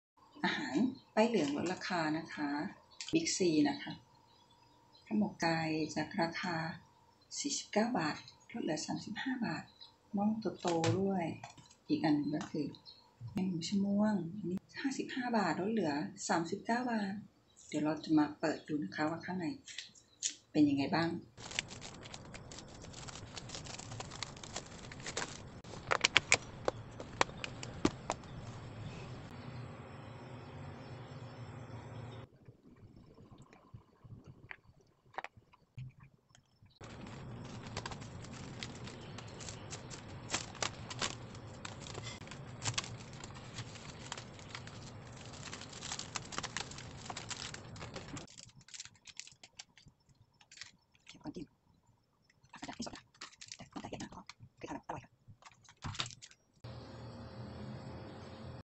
ป้ายเหลืองในห้าง เป็นอีกทางเลือกซื้อในยุคของแพง
รีวิวอาหารป้ายเหลืองในห้างยุคของแพง อิ่มอร่อยแบบประหยัด